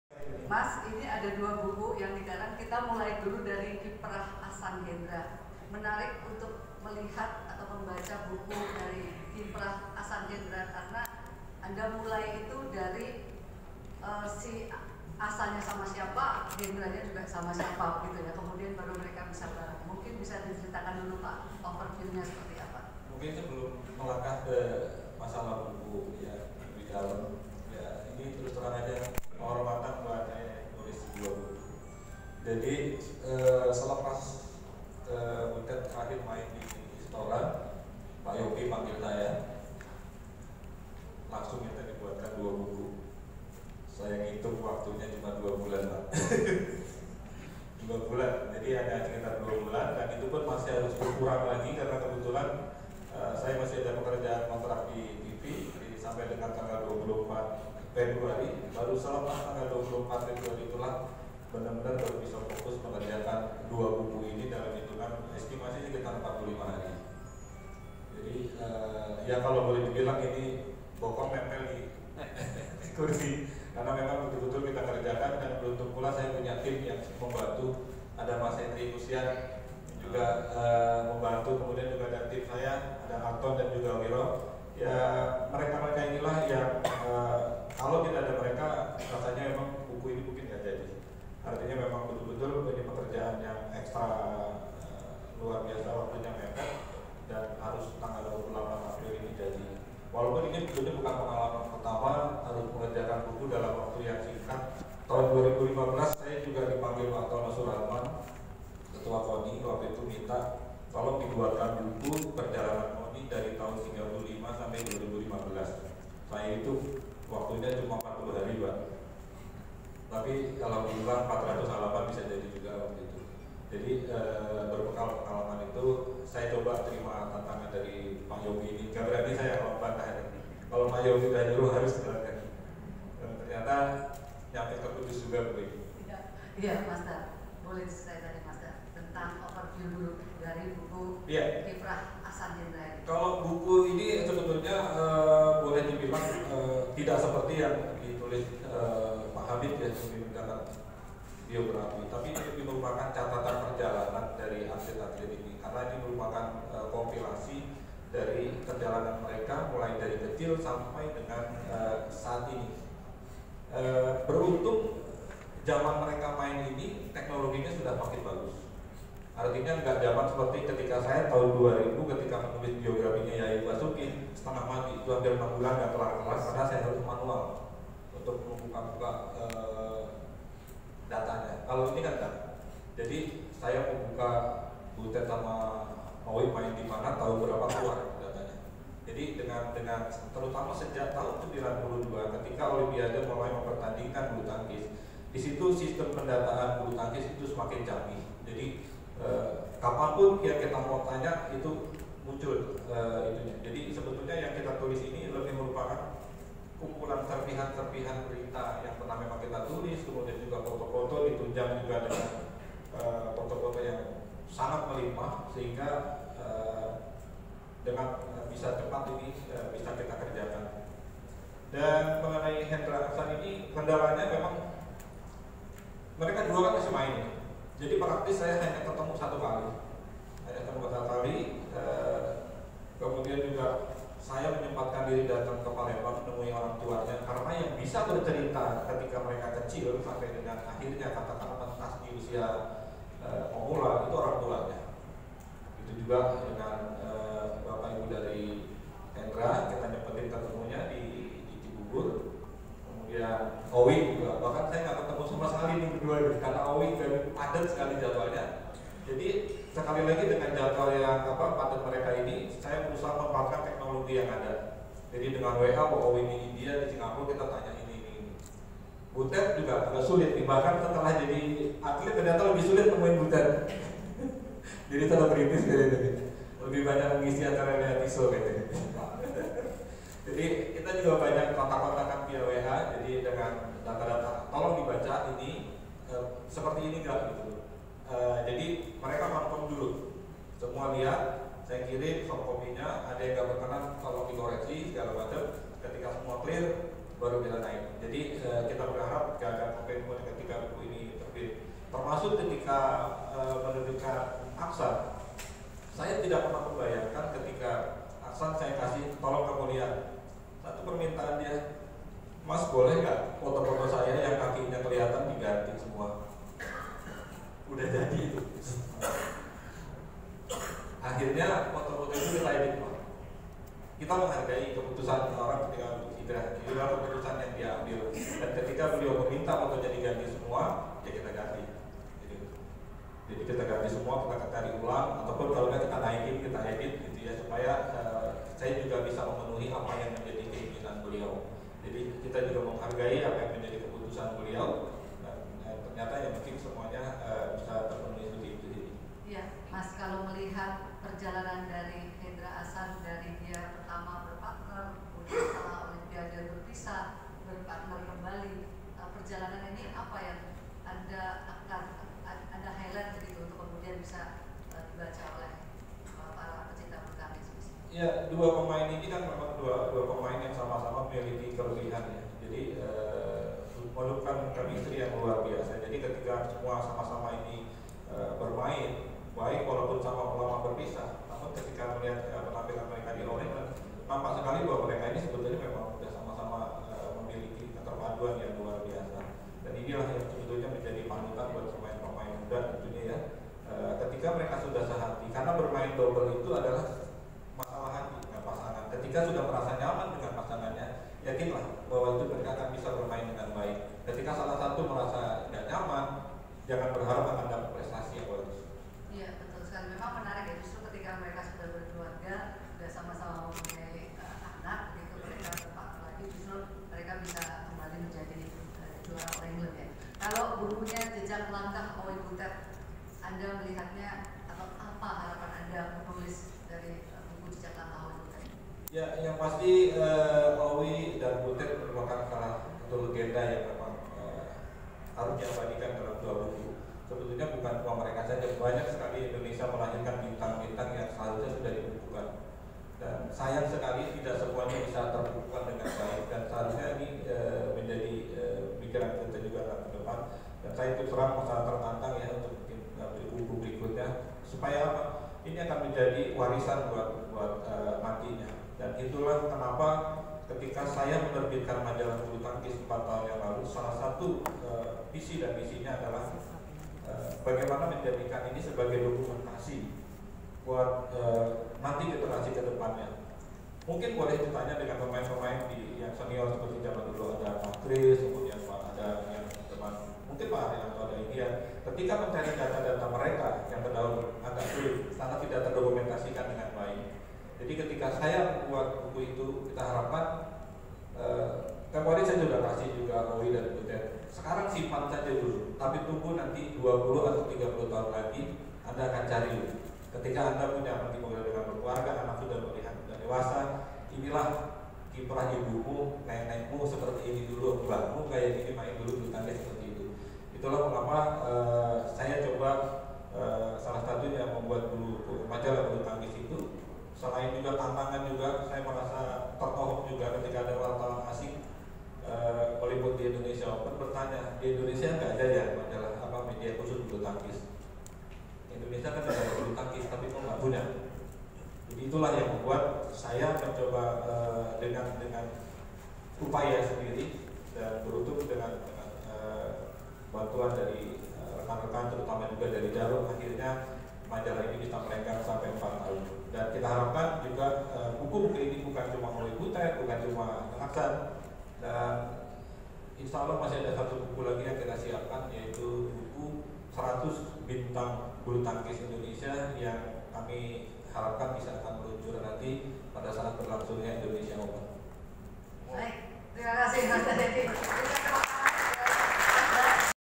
Cerita di Balik Penulisan Buku Owi-Butet dan Ahsan-Hendra
Peluncuran 4 Buku dalam Ulang Tahun PB Djarum ke-50